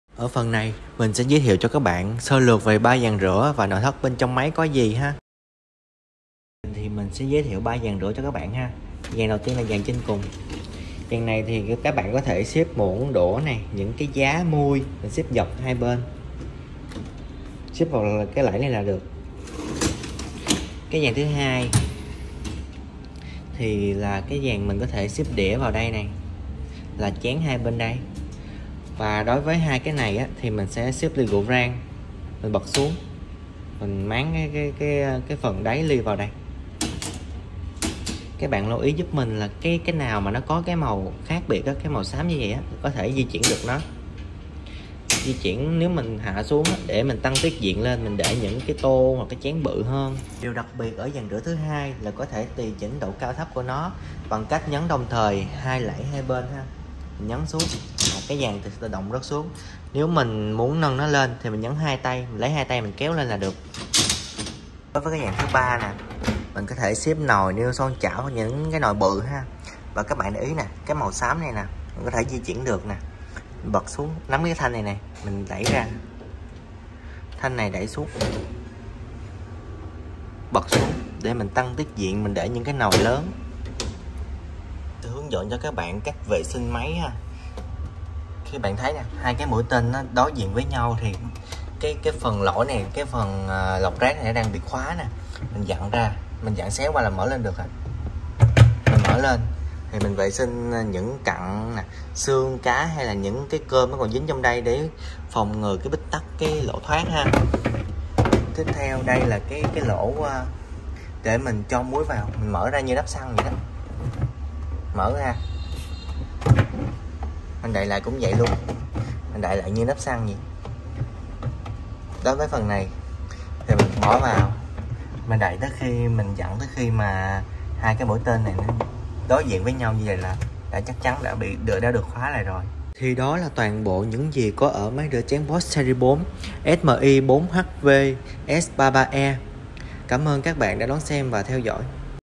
Reviwe chân thật máy rửa bát Bosch bán âm SMI4HVS33E Series 4 - Phần 2
Review  máy rửa chén Bosch bán âm - máy rửa bát bán âm Bosch SMI4HVS33E Seri 4 Phần 2
-Ở Video này Sài Gòn Bếp sẽ giới thiệu đến các bạn mẫu máy rửa chén Bosch bán âm SMI4HVS33E Seri 4 . Với thiết kế hòa vào không gian tủ bếp tạo nên điểm nhấn nổi bật và sang trọng cho không gian bếp gia đình. Máy với sức chứa 13 bộ đồ ăn Châu Âu phù hợp cho 3 - 4 buổi cơm gia đình Việt Nam sẽ là sự lựa chọn tuyệt vời cho bạn.
-Máy được trang bị các tính năng hiện đại như: Homeconnet, rửa 1/2 (rửa nữa tải), sấy tăng cường... Mang đến cảm nhận hoàn hảo cho người sử dụng.